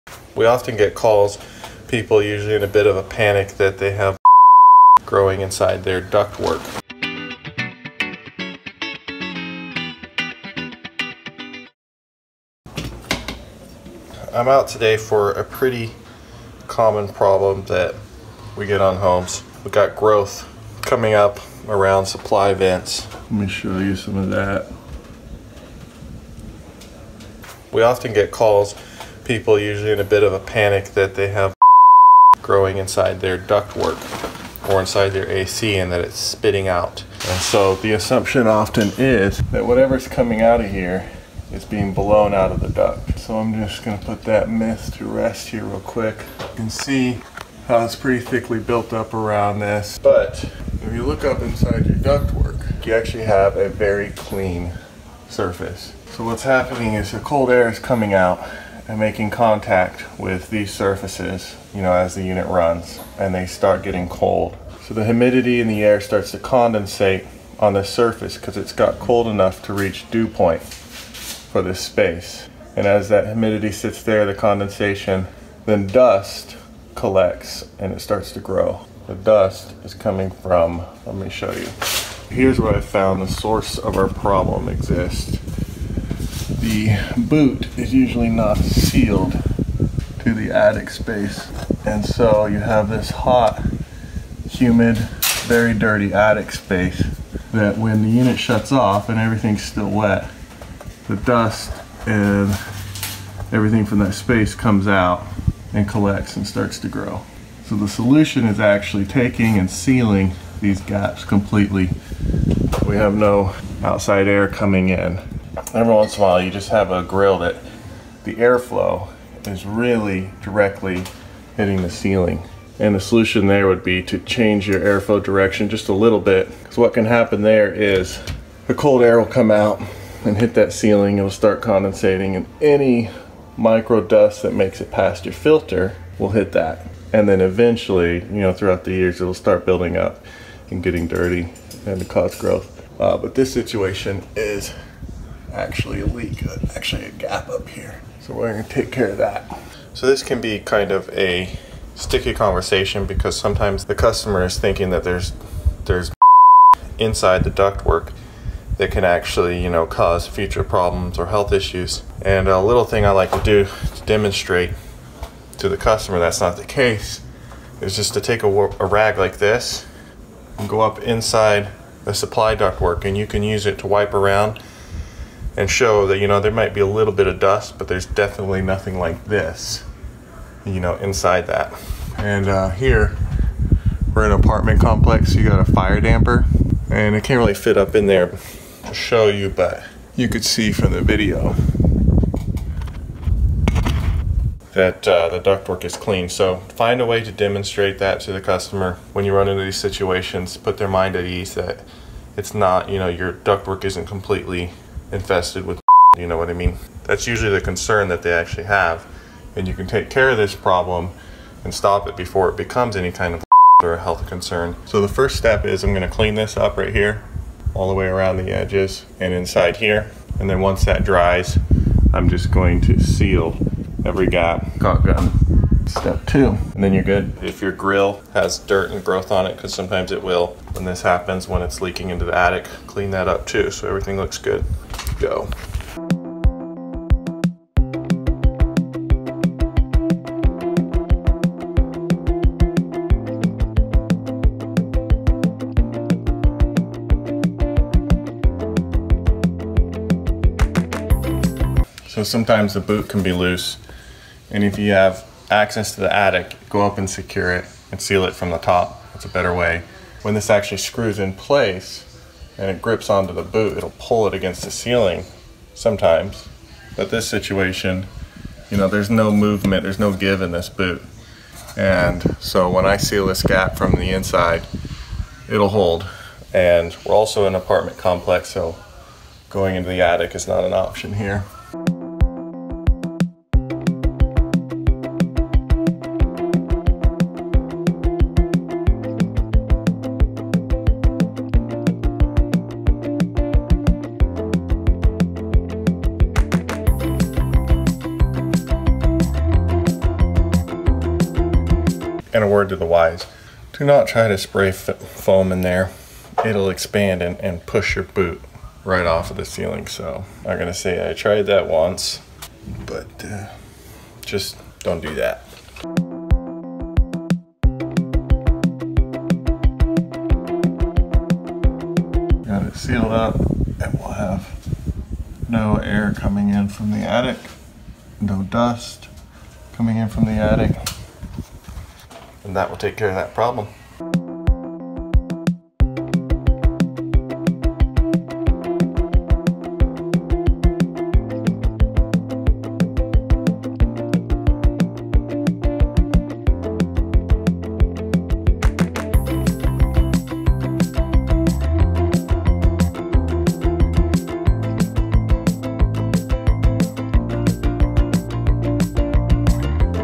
Is There Mold in my Ducks! 🦆(Ducts)
In typical bertlife fashion, Bert answers the age-old customer question: "Is there mold in my ducks!?" He shows why sometimes the mold that's growing on the vent isn’t always on the inside of the ducts.

When there's growth near a supply vent, customers start to feel concerned and wonder if it's inside their ductwork as well. In truth, if you remove the vent and look inside the actual duct, you likely won't see any growth.

When cold air makes contact with the surfaces around the duct, the humidity starts to come out of the air and sit near the vent. As dust and spores from a poorly sealed boot gather on the condensation, fungal growth will happen around the vent. The solution is to seal those gaps.

In some cases, you may also need to change the direction of airflow to prevent growth from forming when condensation happens on the ceiling.

If possible, you'll give the customer some peace of mind by reaching inside the ductwork, wiping the walls with a rag, and showing the customer that there is no mold on the rag.

Bert starts by cleaning around the vent, and then he seals all of the gaps with caulk. Sealing around the boot with spray foam is NOT a good idea because it expands and may displace the boot.